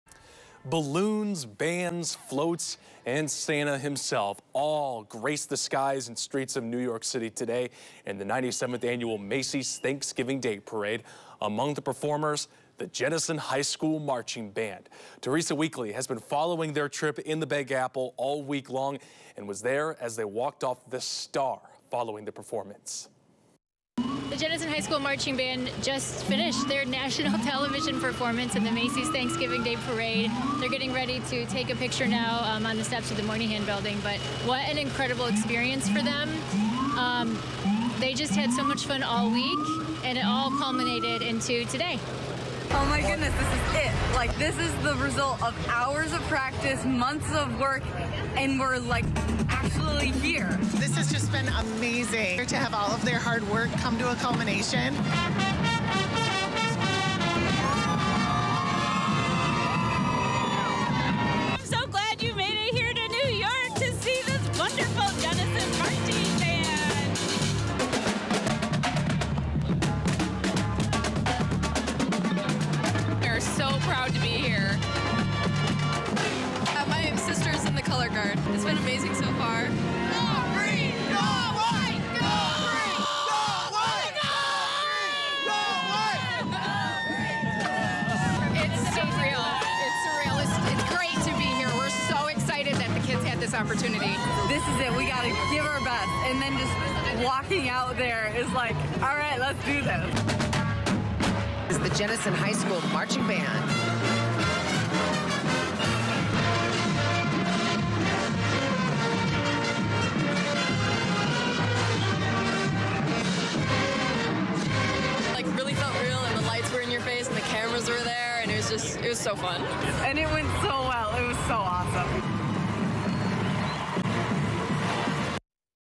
Fans from MI lined NYC parade route to cheer on Jenison students
Students in the Jenison High School marching band are still feeling a sense of euphoria as they spend thanksgiving in New York City, after a morning that included time on national television. (Nov. 22, 2023)